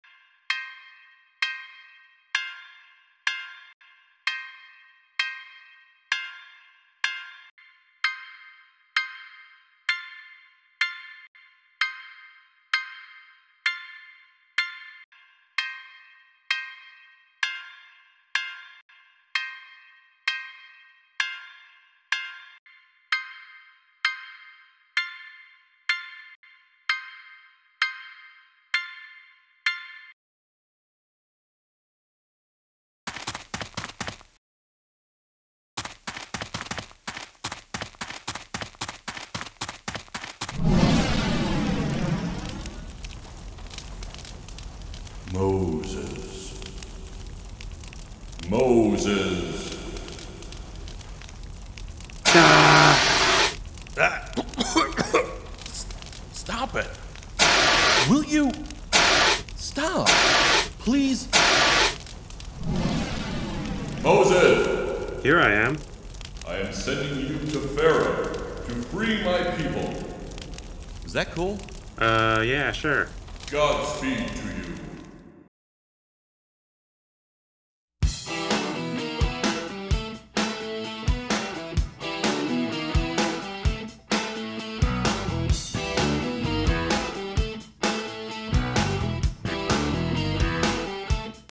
VBS 2012 Day 1 Moses and the Burning Bush
It's that time of year again.  Part one of the five part VBS series.